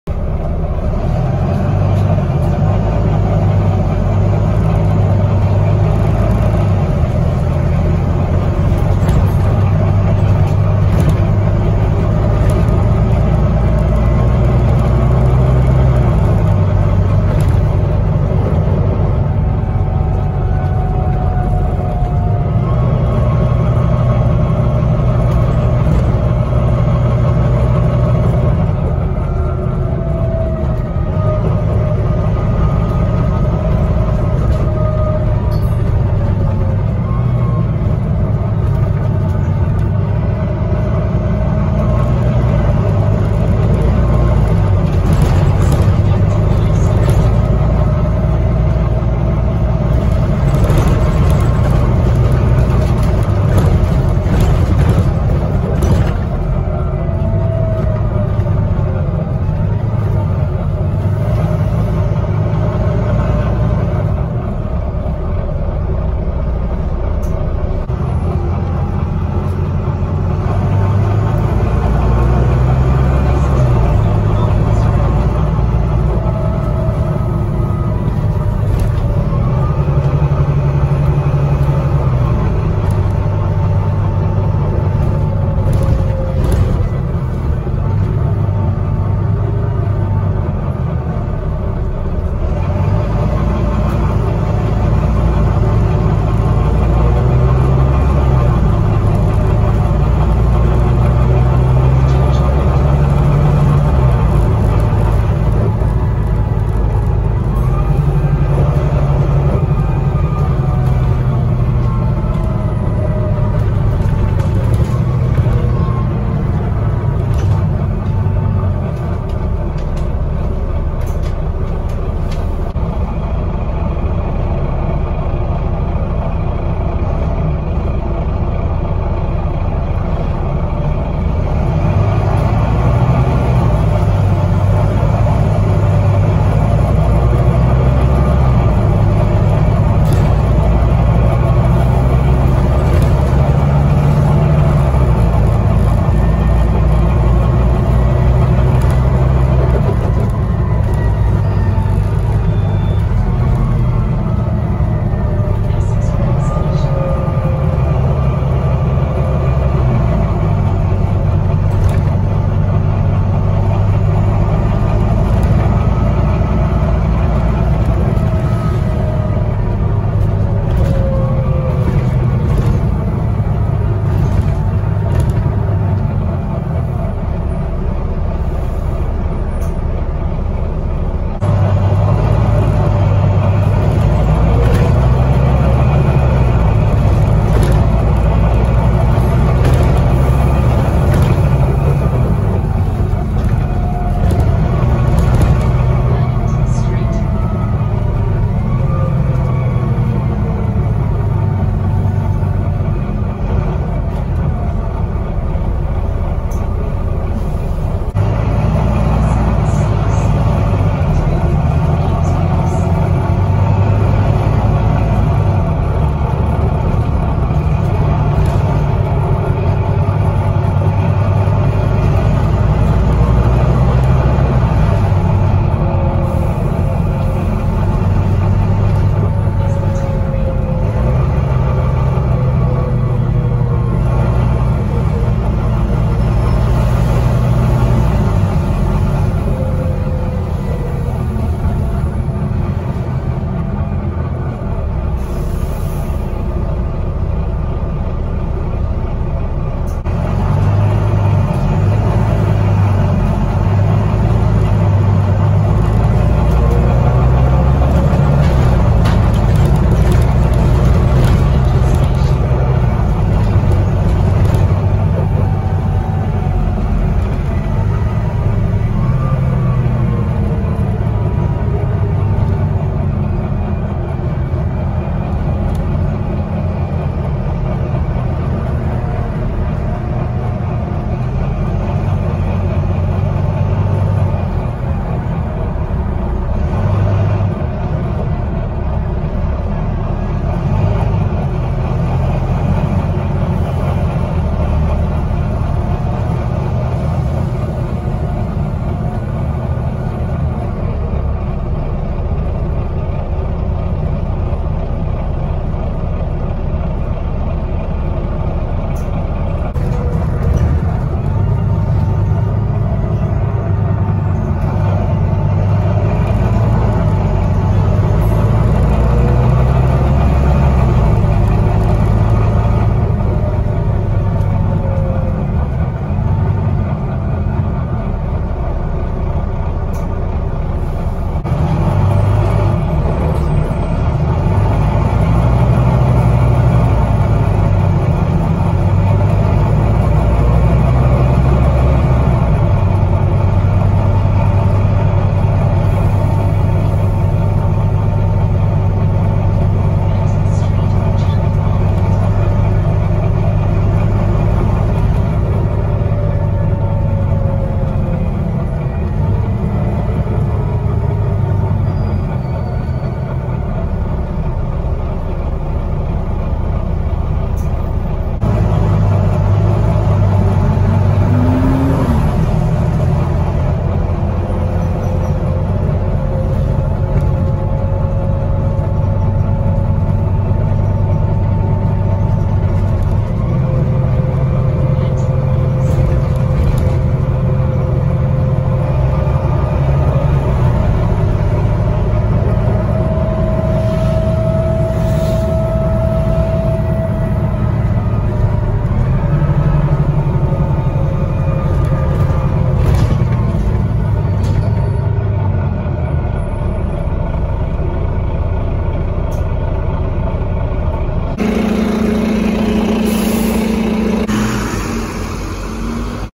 FLOORED | Journey on route 476 (WVN11) (LK59FEO) | Go Ahead London General
From my channel name you can tell that this is my favourite bus route and it always has since it had plaxton presidents on it.

I have no idea what the future is looking like for these buses allocated on the route but it would make sense for NP to allocate these to the 67 if they’re going to allocate Ee’s to the 476 as most of the green blinded WVNs have 67 blinded as they used to make appearances on it before it was with Arriva.  However, knowing NP, I feel like this won’t even happen.

These WVNs do get floored, especially in the night, but I stayed on this bus until King’s Cross but I only recorded where the driver was driving quite fast so I hope you enjoy.